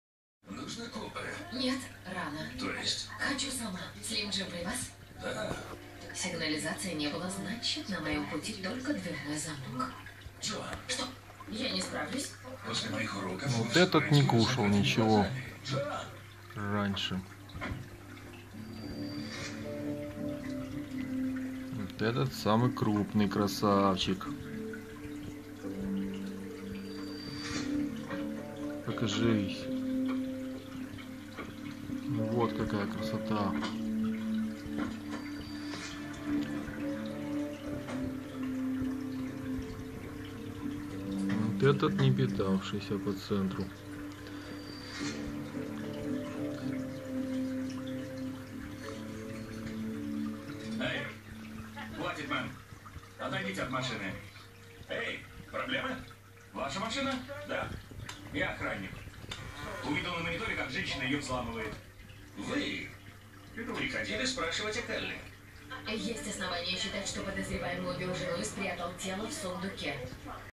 Мой видовой аквариум
Перуанский альтум, велифера снежинка, анциструсы СП, гуппи испанцы.